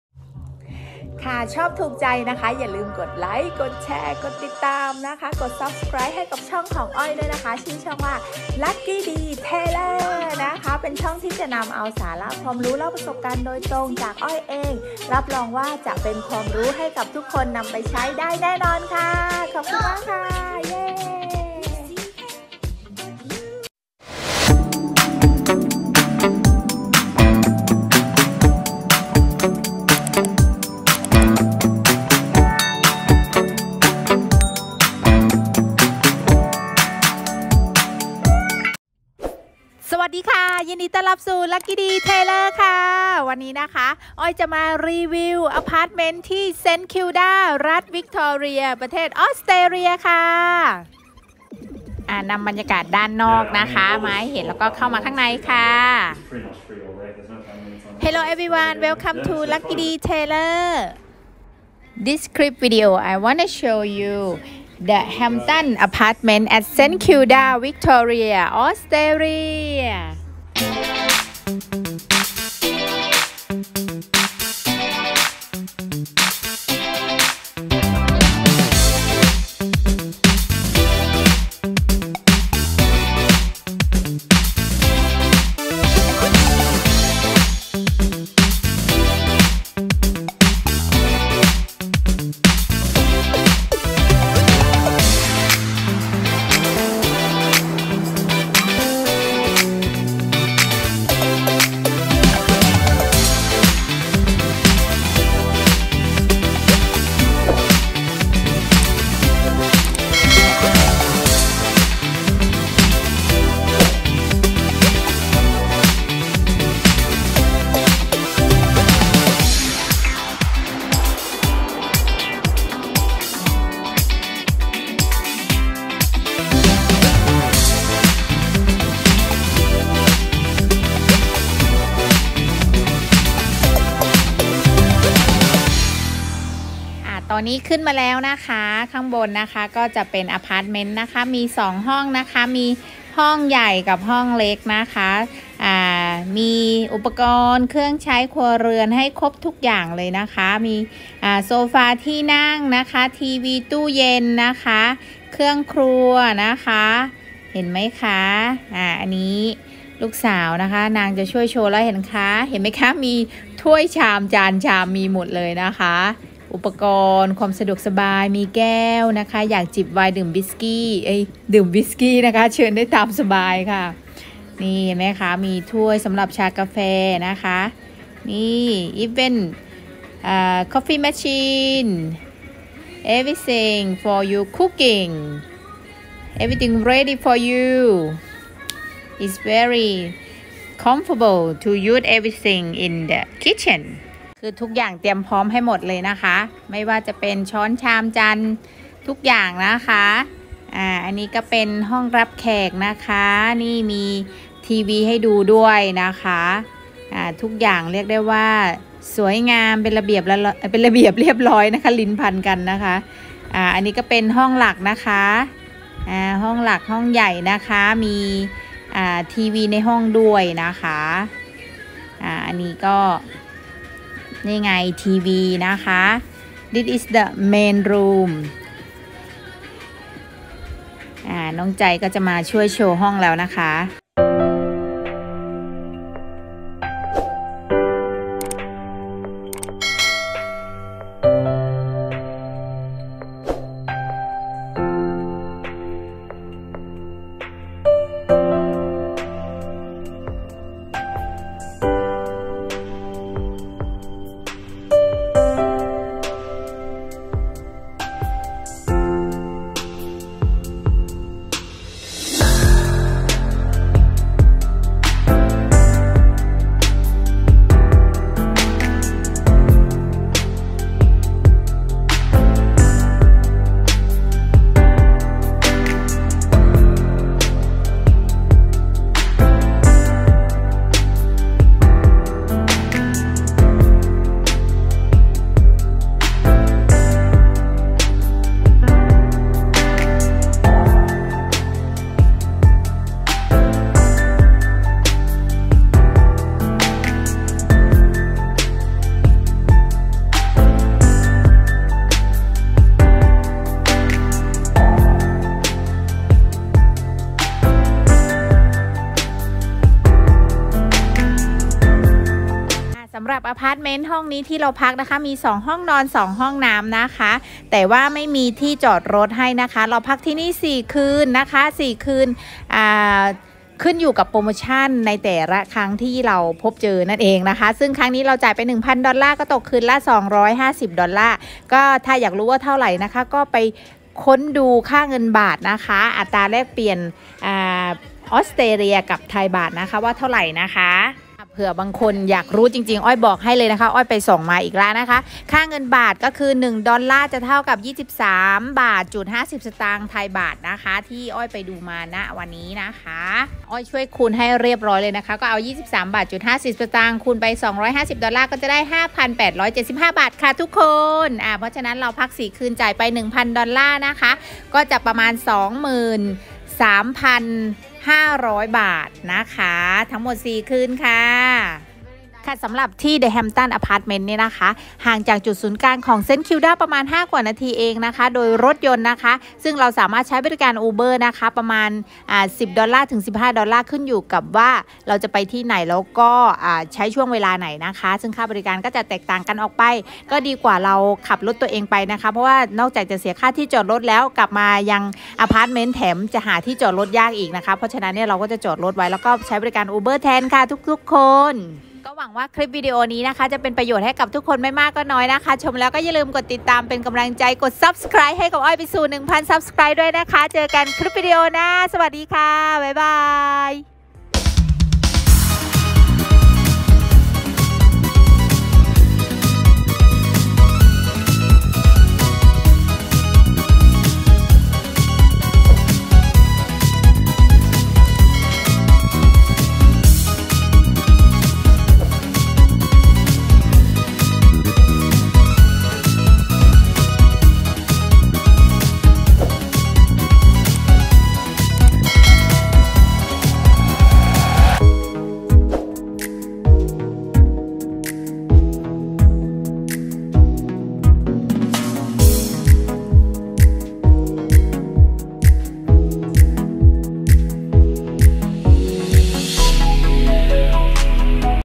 The Hamptonts Apartments St.Kilda Australian 2022#รีวิวห้องพักที่เมืองSt.Kilda ออสเตรเลีย
สวัสดีค่ะคลิปวีดีโอนี้อ้อยจะพาไปชมบรรยากาศห้องพักเป็นรูปแบบอพาร์ตเมนต์ที่เมืองSt.Kilda รัฐวิคตอเรียประเทศออสเตรเลียค่ะ ชมแล้วก็อย่าลืมกดติดตามเป็นกำลังใจให้อ้อยด้วยค่ะ ขอบคุณค่ะ🙏😄❤️